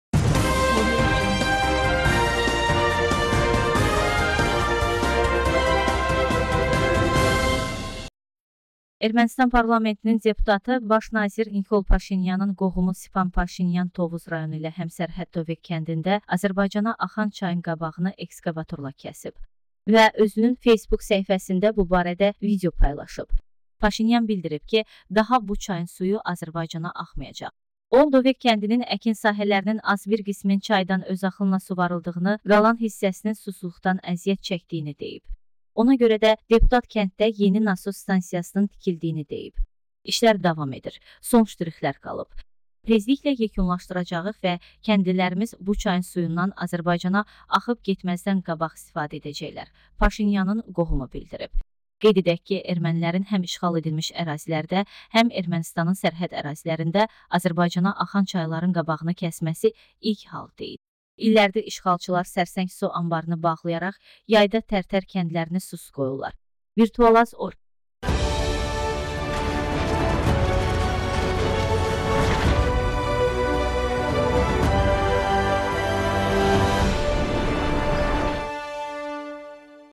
ermenistan Azərbaycana axan çayın qabağını kəsdi
Ermənistan parlamentinin deputatı, baş nazir Nikol Paşinyanın qohumu Sipan Paşinyan Tovuz rayonu ilə həmsərhəd Doveq kəndində Azərbaycana axan çayın qabağını ekskovatorla kəsib. Və özünün Facebook səhifəsində bu barədə video paylaşıb.

Paşinyan bildirib ki, daha bu çayın suyu Azərbaycana axmayacaq.

O, Doveq kəndinin əkin sahələrinin az bir qisminin çaydan özaxınla suvarıldığını, qalan hissəsinin susuzluqdan əziyyət çəkdiyini deyib. Ona görə də deputat kənddə yeni nasos stansiyasının tikildiyini deyib. “İşlər davam edir, son ştrixlər qalıb. Tezliklə yekunlaşdıracağıq və kəndlilərimiz bu çayın suyundan Azərbaycana axıb getməzdən qabaq istifadə edəcəklər”,-Paşinyanın qohumu bildirib.

Qeyd edək ki, ermənilərin həm işğal edilmiş ərazilərdə, həm Ermənistanın sərhəd ərazilərində Azərbaycana axan çayların qabağını kəsməsi ilk hal deyil. İllərdir işğalçılar Sərsəng su anbarını bağlayaraq yayda Tərtər kəndlərini susuz qoyurlar.